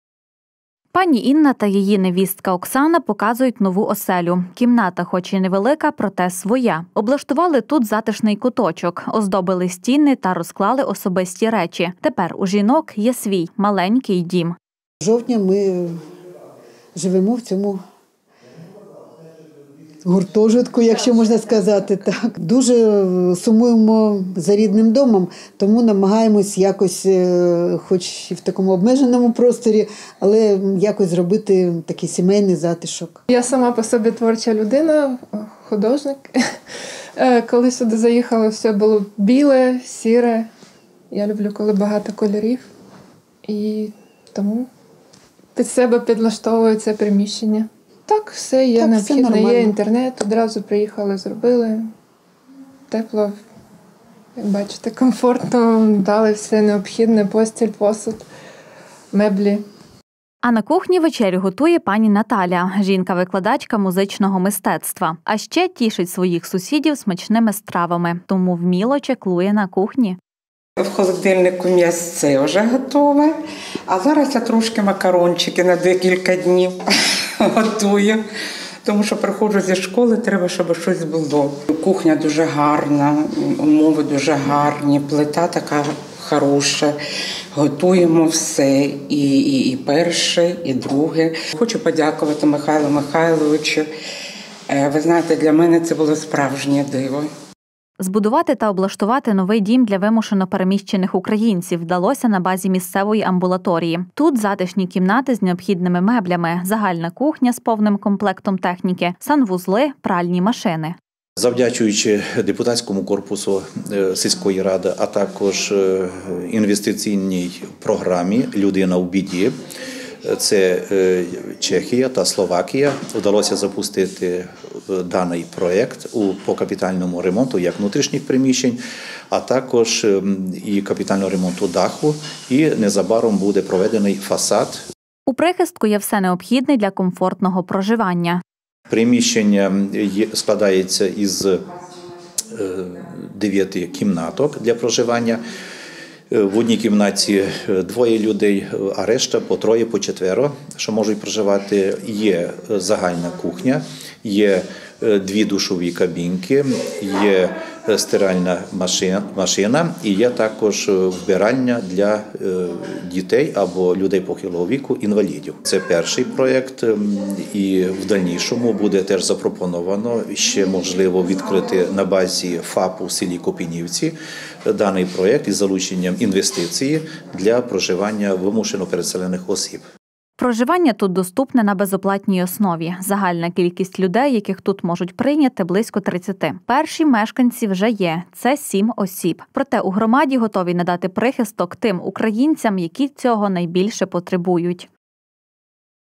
Подарували другий дім
Пані Інна та її невістка Оксана показують нову оселю. Кімната хоч і не велика, проте своя. Облаштували тут затишний куточок, оздобили стіни та розклали особисті речі. Тепер у жінок є свій маленький дім. На кухні вечерю готує пані Наталя. Жінка викладачка музичного мистецтва. А ще тішити своїх сусідів смачними стравами. Тому вміло чаклує на кухні. Збудувати та облаштувати новий дім для вимушено переміщених українців вдалося на базі місцевої  амбулаторії. Тут затишні кімнати з необхідними меблями, загальна кухня з повним комплектом техніки, санвузли, пральні машини. У прихистку є все необхідне для комфортного проживання. Проживання тут доступне на безоплатній основі. Загальна кількість людей, яких тут можуть прийняти - близько 30. Перші мешканці вже є  - це 7 осіб. Проте у громаді готові надати прихисток тим українцям, які цього потребують.